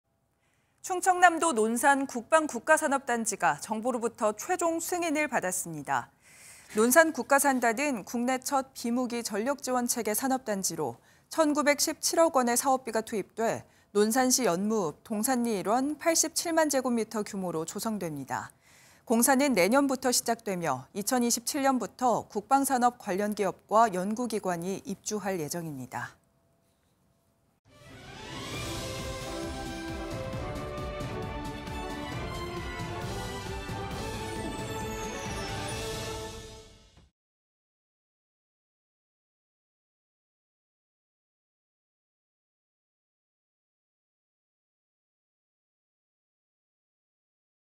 논산 국방국가산업단지 최종 승인…2027년부터 기업 입주 [전국네트워크]
충청남도 논산 국방국가산업단지가 정부로부터 최종 승인을 받았습니다.

  논산 국가산단은 국내 첫 비무기 전력지원체계 산업단지로, 1천917억 원의 사업비가 투입돼 논산시 연무읍 동산리 일원 87만 제곱미터 규모로 조성됩니다.

  공사는 내년부터 시작되며 2027년부터 국방산업 관련 기업과 연구기관이 입주할 예정입니다.